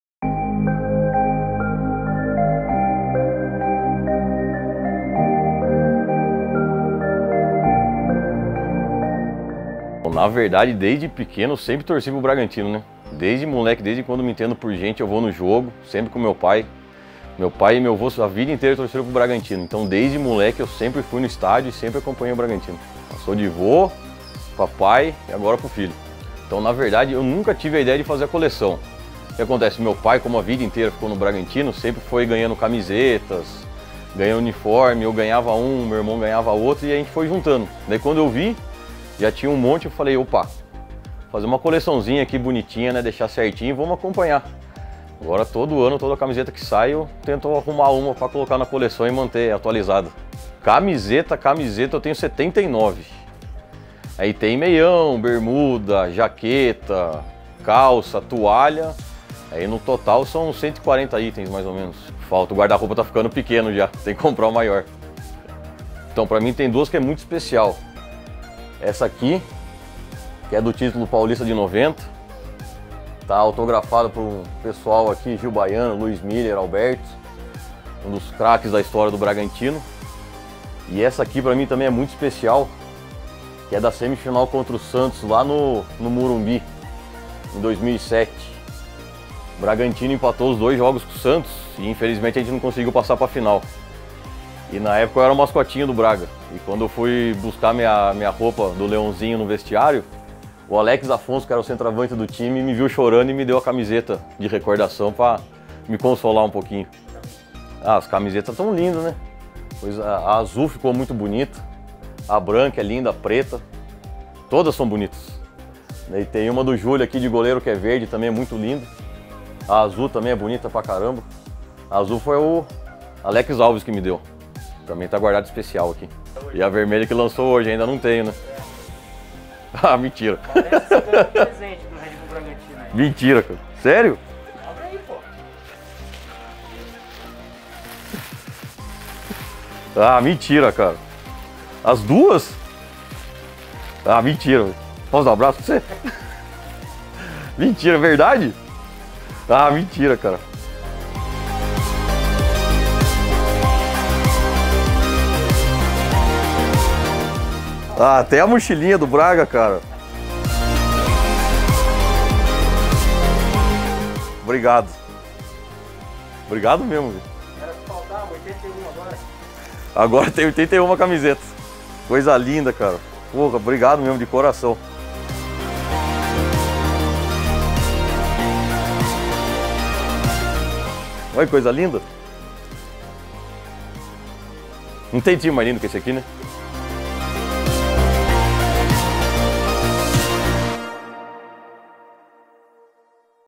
Amor à flor da (segunda) pele
Conversamos com Rafael L. Andrade, torcedor do MassaBruta que coleciona camisas da equipe e já tem mais de 80 na coleção.